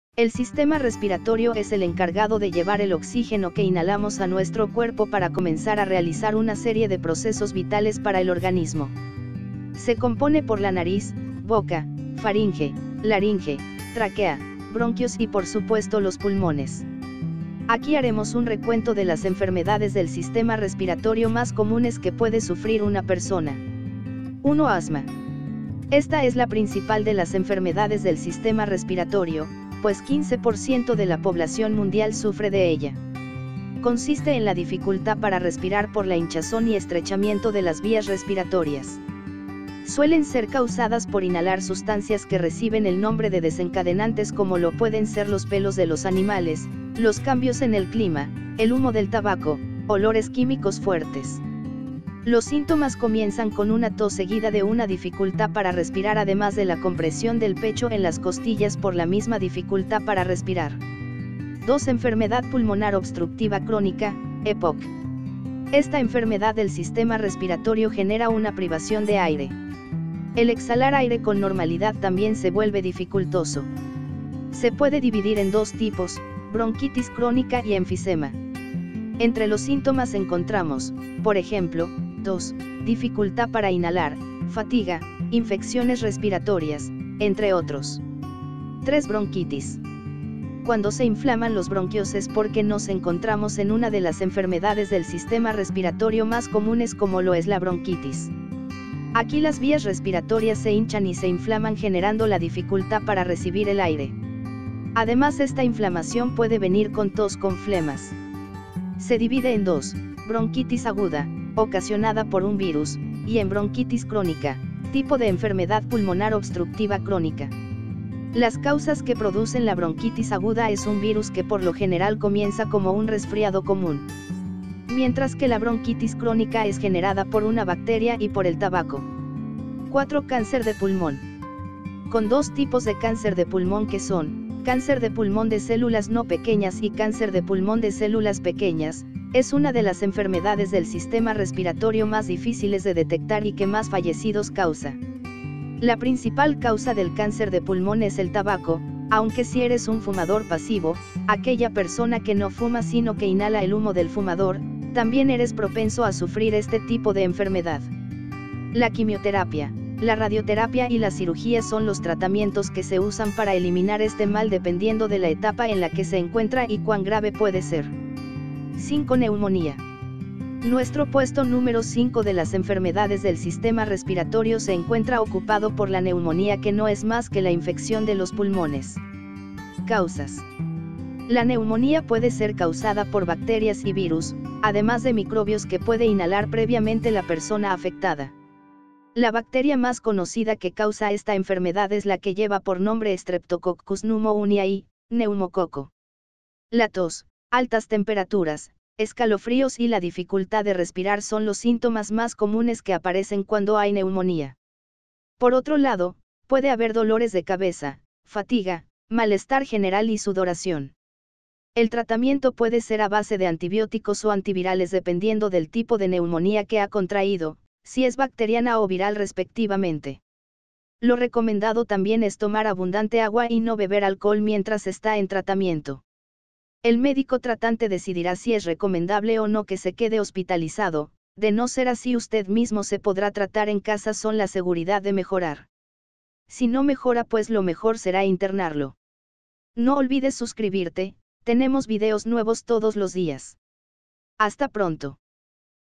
ENFERMEDADES DEL SISTEMA RESPIRATORIO: Causas, Síntomas y Tratamientos
¿Conoces las principales enfermedades del sistema respiratorio?
En este video te contamos las causas, síntomas y tratamiento de las enfermedades del aparato respiratorio.

¡SÍGUENOS!

PUNTUACIÓN DE ESTE ARTÍCULO: ★★★★★/5

▶ Enfermedades del aparato respiratorio más comunes

★ 1. Asma: Es la dificultad para respirar debido a la hinchazón y estrechamiento de las vías respiratorias.
★ 2. Enfermedad Pulmonar Obstructiva Crónica (EPOC): Produce una privación de aire. El espirar aire con normalidad asimismo se vuelve difícil. Se puede dividir en 2 tipos: bronquitis crónica y enfisema.
★ 3. Bronquitis: Las vías respiratorias se llenan y se inflaman produciendo la complejidad para percibir el aire. Además de esto esta inflamación puede venir con tos con flemas
★ 4. Cáncer de pulmón: Es una de las enfermedades del aparato respiratorio más difíciles de hallar y debido a esto causa muchos fallecidos al año.
★ 5. Neumonía: Causado por el virus neumococo o como se le conoce por su nombre científico: Streptococcus pneumoniae.

▶ Órganos que componen el sistema respiratorio:

★ 1. Nariz: vía natural de acceso del aire al interior del organismo: en su paso por las fosas nasales, el aire es purificado, calentado y humedecido a fin de que llegue a los pulmones en unas condiciones ideales.
★ 2. Boca: vía secundaria de entrada del aire, mas esencial, por el hecho de que interviene en la emisión de la voz.
★ 3. Faringe: conducto que desciende tras las fosas nasales y la nariz, genuino cruce de las vías respiratorias y digestibles.
★ 4. Laringe: conducto ubicado entre la faringe y la tráquea que, aparte de ser parte de las vías aéreas, forma el órgano de la fonación.
★ 5. Tráquea: conducto cartilaginoso, de diez-quince cm de longitud, ubicado entre la laringe y el origen de los bronquios.
★ 6. Bronquios: conductos resultantes de la bifurcación de la tráquea, que se marchan ramificando en otros, considerablemente más finos, llamados bronquiolos.
★ 7. Pulmones: órganos básicos del aparato respiratorio, situados dentro de la caja torácica, que se hacen cargo del intercambio de gasesentre el aire y la sangre.

Mejor con salud y cuidado, siguiendo los consejos previamente del video o que se encuentran en el artículo.